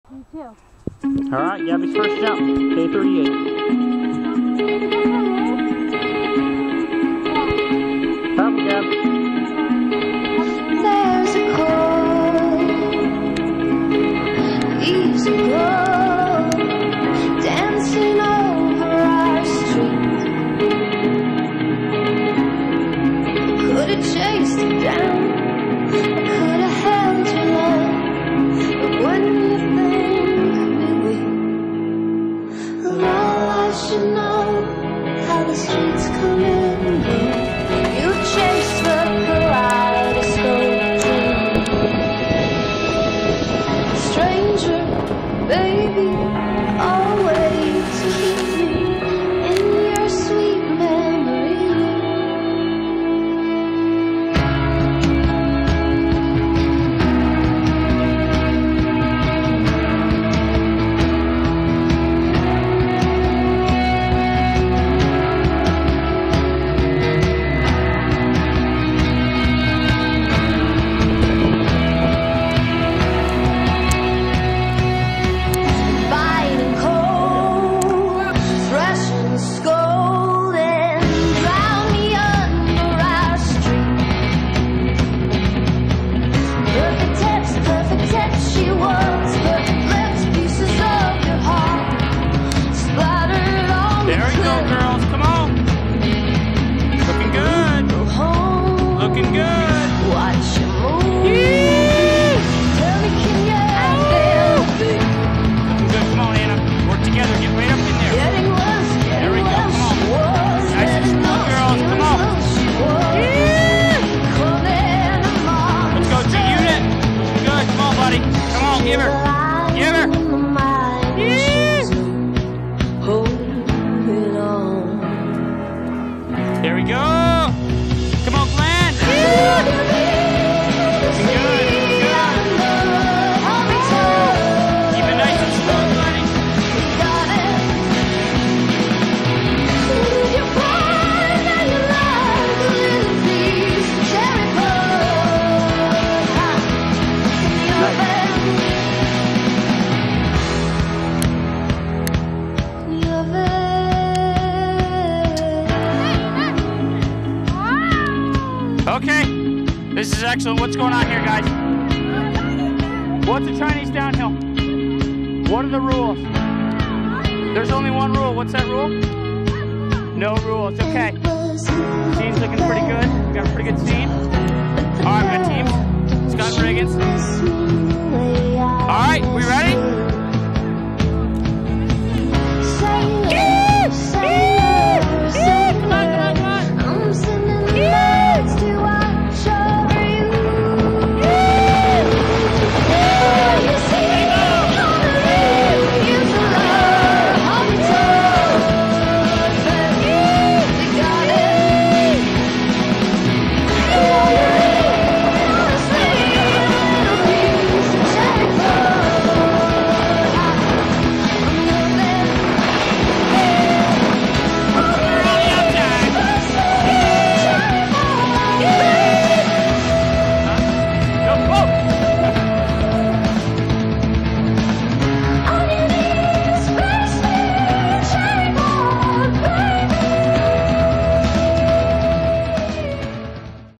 Team Grasshopper-- Rippin' Into 2011
Highlights of NYSEF Team Grasshopper's foray into 2011. Some Chinese downhill, trip to Andover NH, New Year's Competition and XC/NC races, and some adventures along the way.